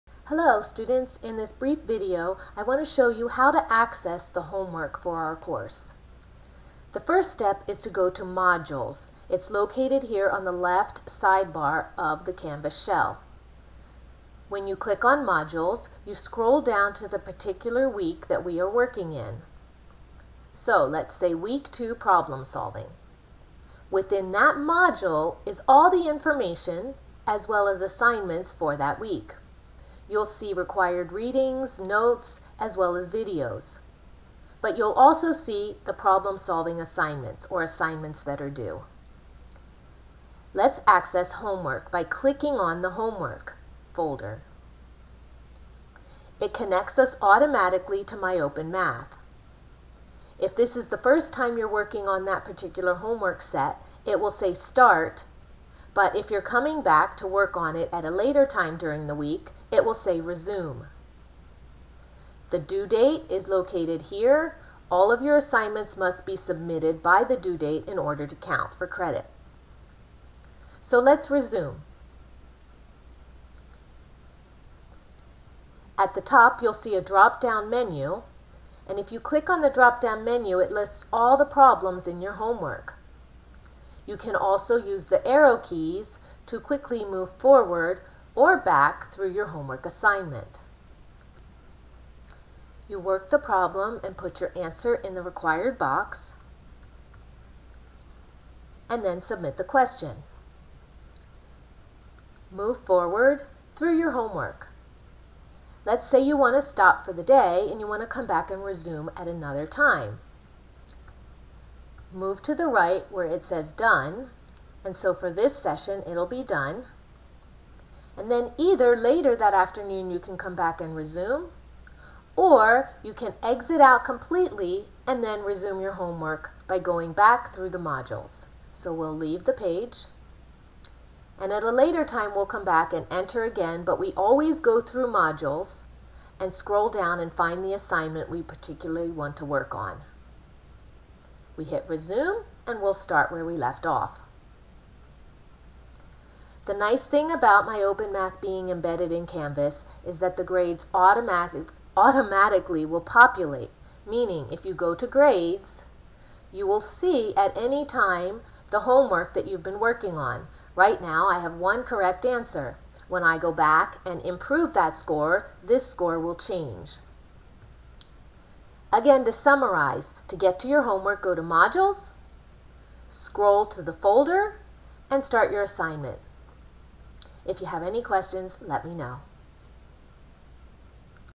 Accessing MyOpenMath Homework in Canvas
This brief video shows students how to access and complete homework in Canvas using MyOpenMath integration.